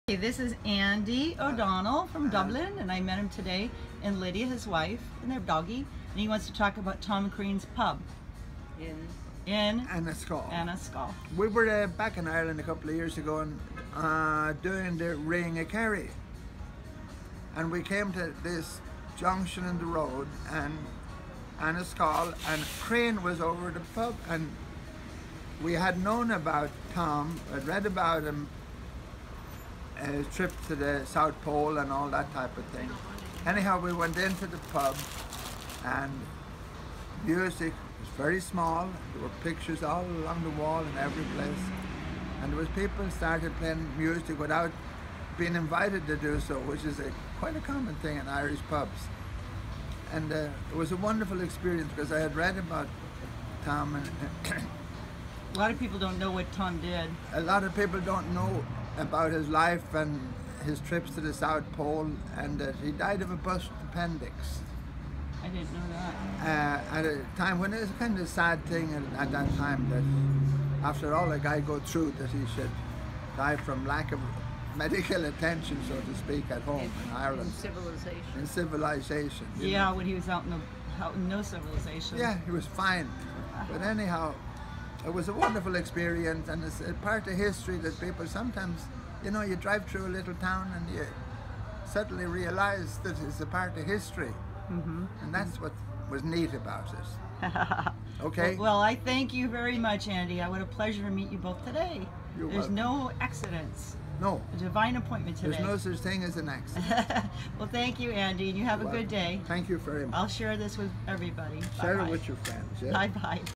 Andy discuss Tom Crean explorer and Crean pub
Being a descendant of the Antarctic explorer Tom Crean, ( Shackleton Adventure) I'm always interested in hearing stories from admirers.

I met Andy from Dublin today. Recognized his brogue.  One thing lead to another the subjects of pubs then Tom Crean came up so I asked if I could video and share this.